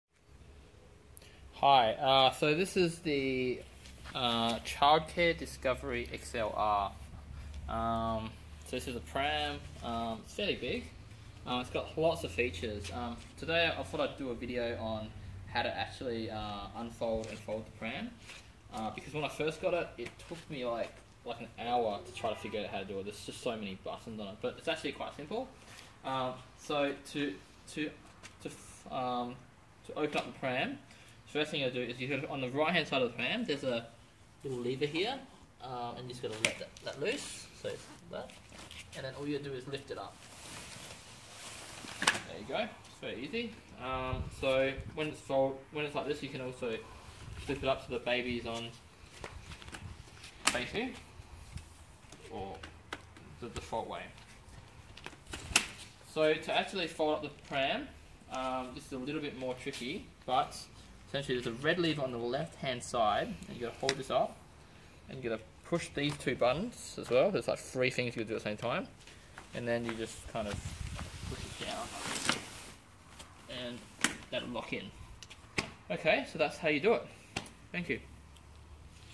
How to fold a pram / stroller - Childcare Discovery XLR Stroller
This is the Childcare Discovery XLR Stroller. We got this as a gift and couldn't figure out how to actually fold and unfold it! Quite easy once you know what to do - but there's so many buttons that it's quite hard to figure it out the first time.